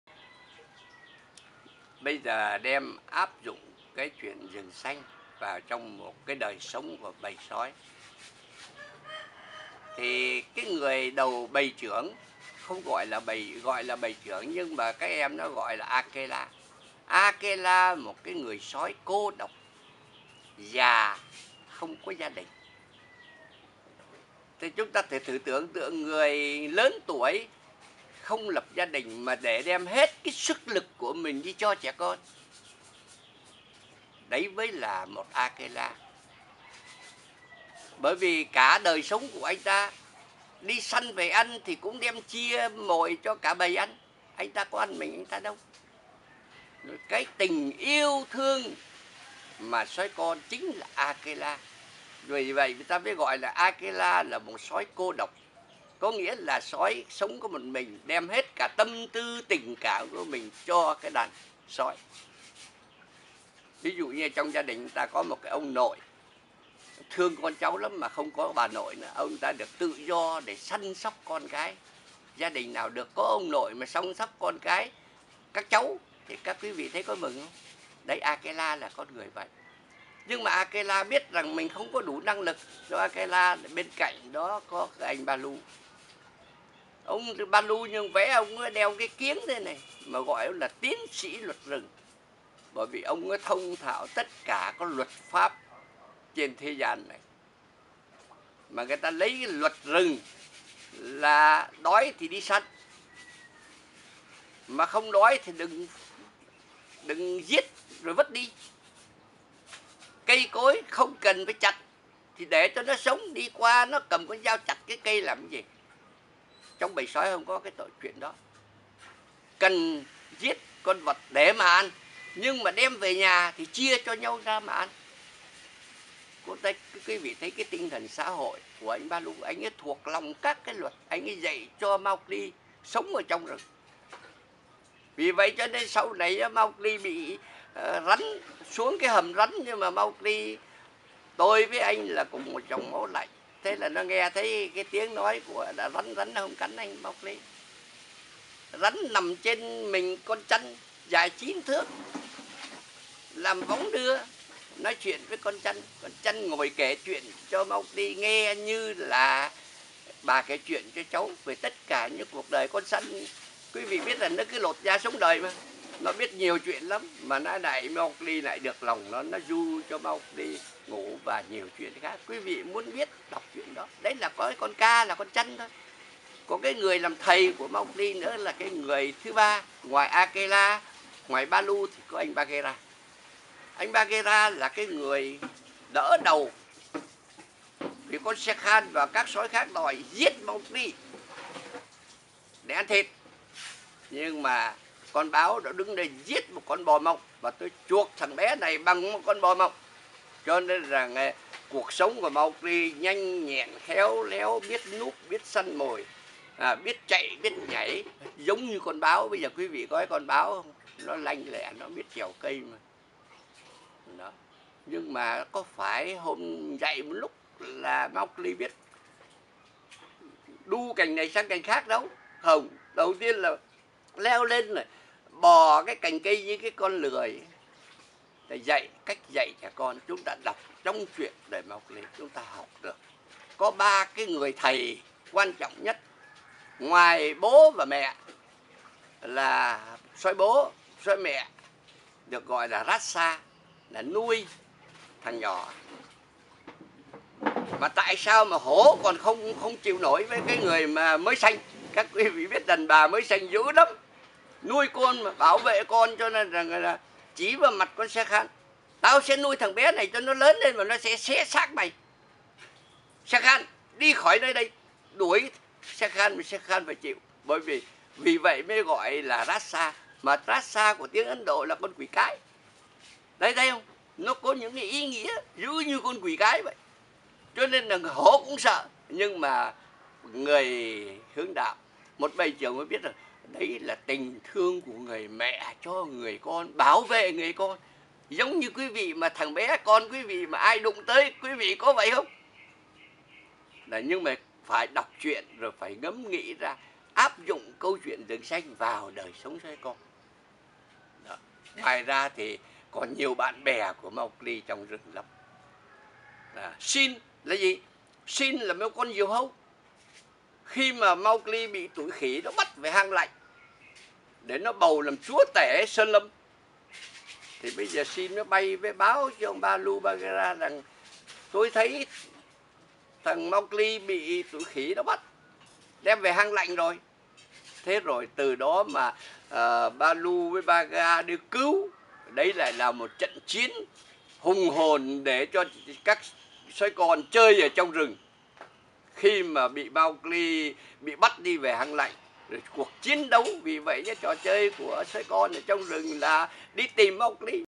CÁC NHÂN VẬT RỪNG. Tr. Trần Văn Hiến
Câu chuyện Rừng Xanh của Rudyard Kipling làm bối cảnh và là phương pháp giáo dục các em Sói con